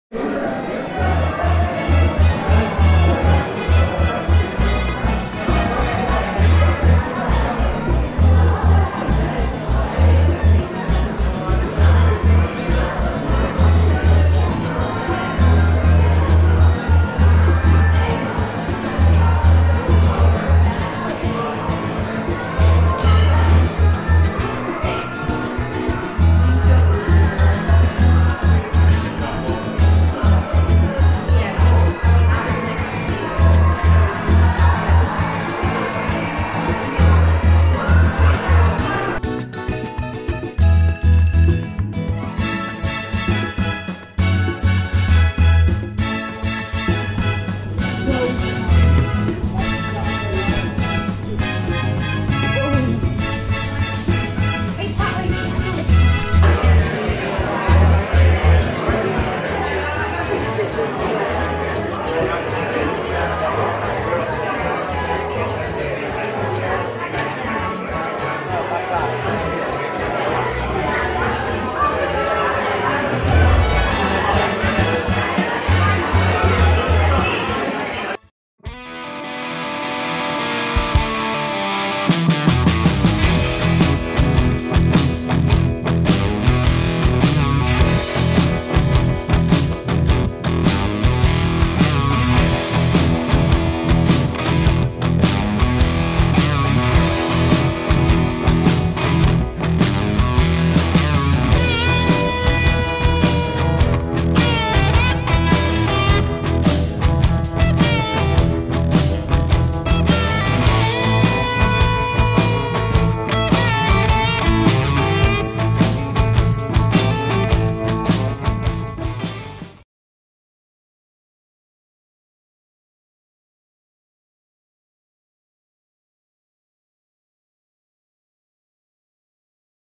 Back at the day job, wedding catering.
After an amazing time in Mexico, I am now back in the UK doing one of  the day jobs.